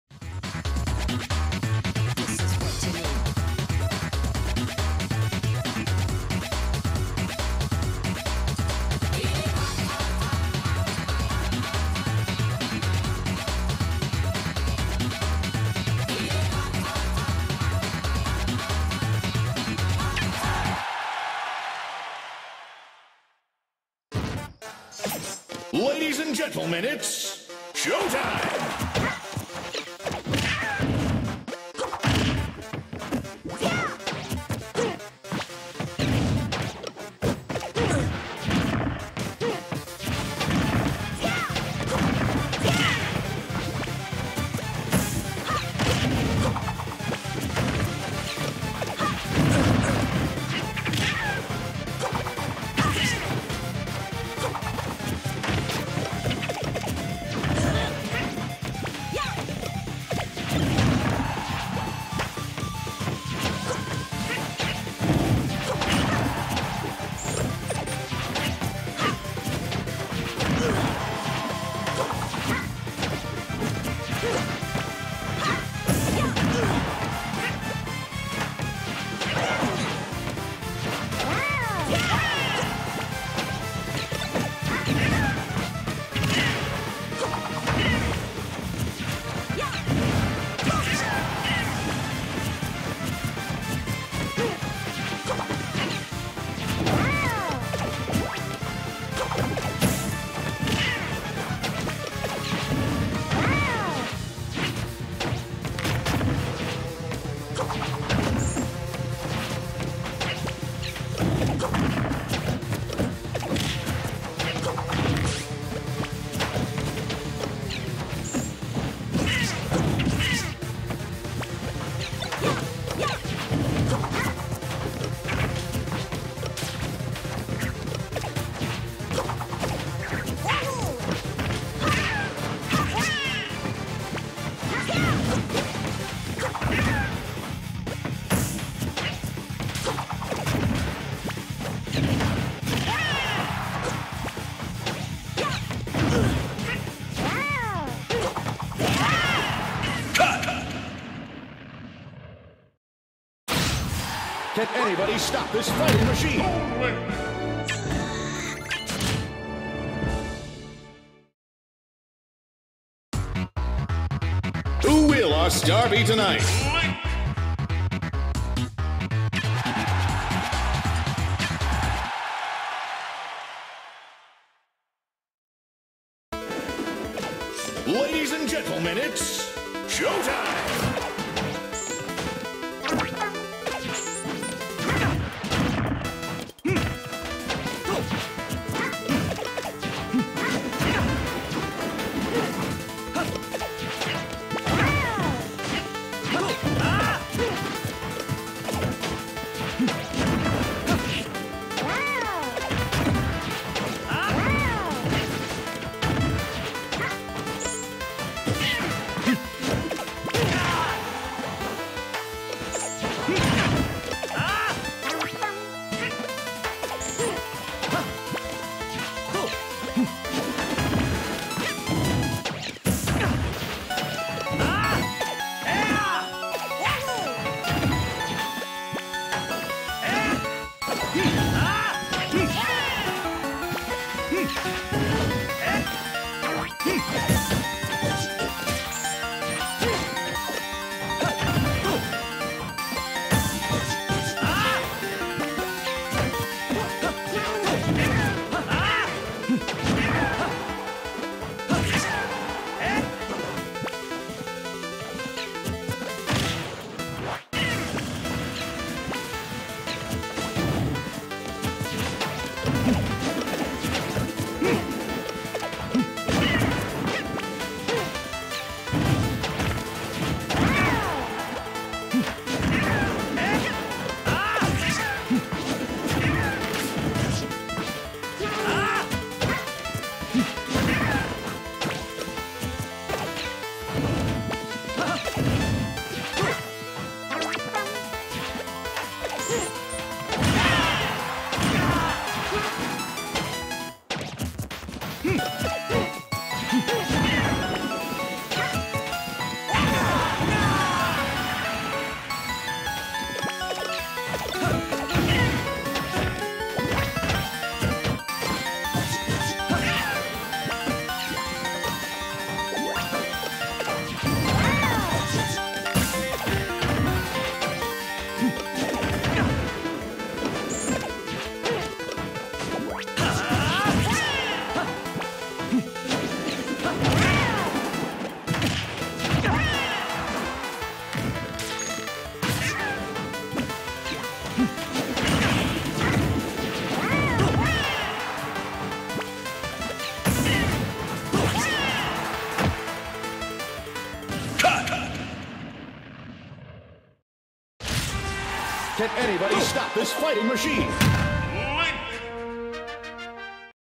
Clarkson's Battlefield 14 – Winners DD | Unshorn (Yoshi) vs HBA | Glentendo (TL, Link)
Titles and editing by Amanda Rae Rosado